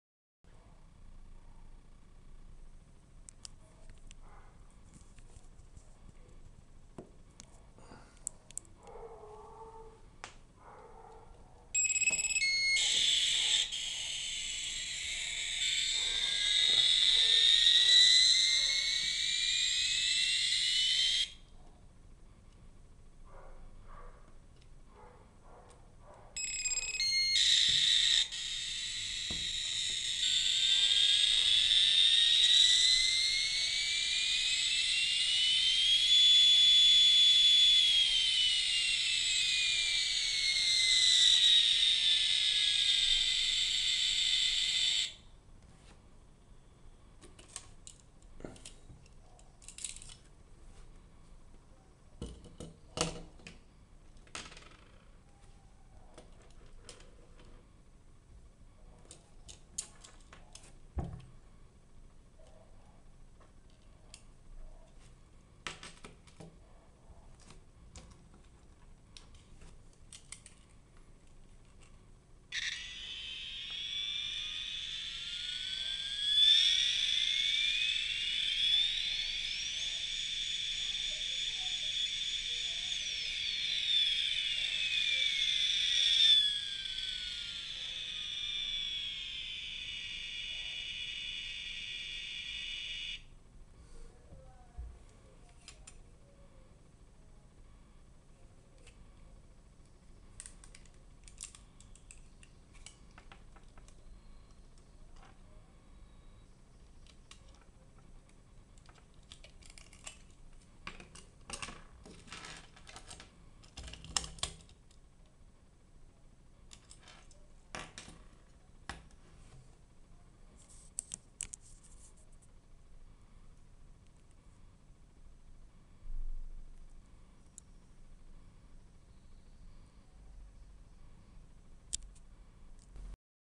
K and K - VTG 80's TOMCAT F 14 JET FIGHTER PLANE BATTERY OPERATED KEY CHAIN
A great treasure from the past, these F-14 fighters were a must have accessory back then! With sound or lights and sound features, this is an impressive key chain even now! Watch the video and enjoy! You can buy these along with many other key chains here: